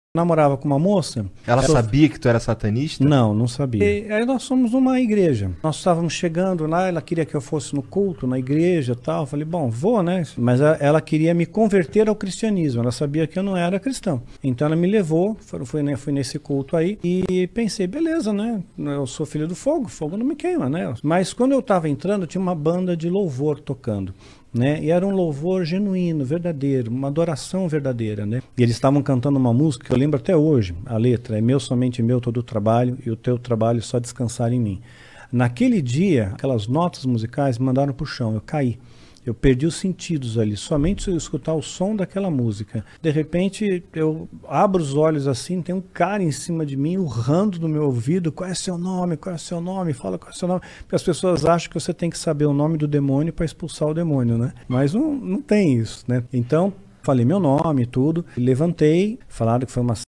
Ex satanista Daniel Mastral é encontrado morto em sua residência. Ele abandonado a seita pagã
FALOU DEMAIS - ex satanista Daniel Mastral é encontrado morto em sua residência. Ele havia abandonado a seita pagã e se convertido ao EvangelhoO escritor e Pastor evangélico Daniel Mastral foi encontrado morto na tarde deste último domingo (4),